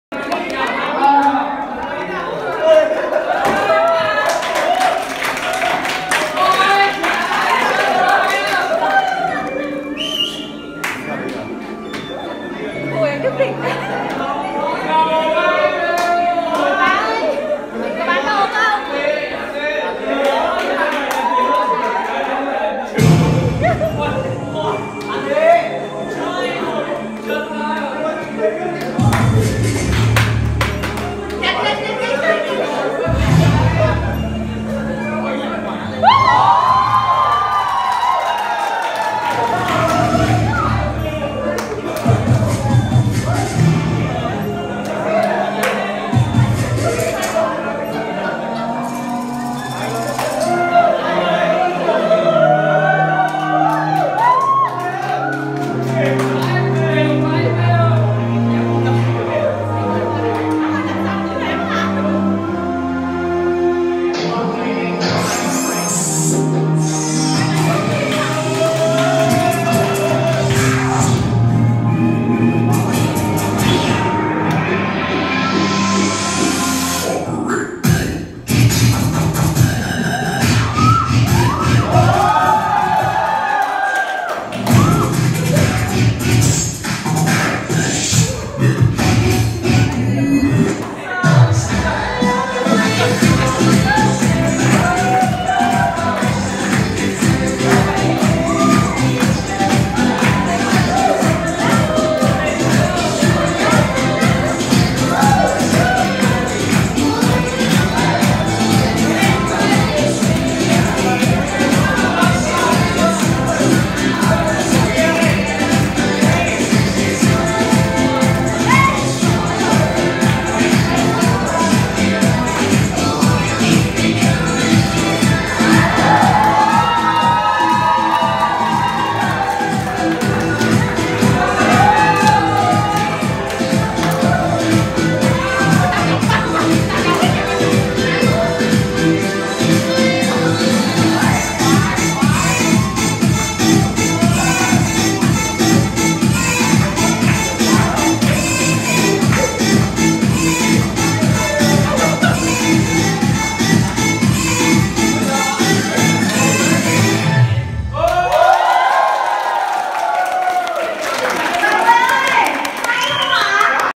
Nhảy Poping Chúc Mừng 20/10
Tiết mục được biểu diễn bởi Anh Đức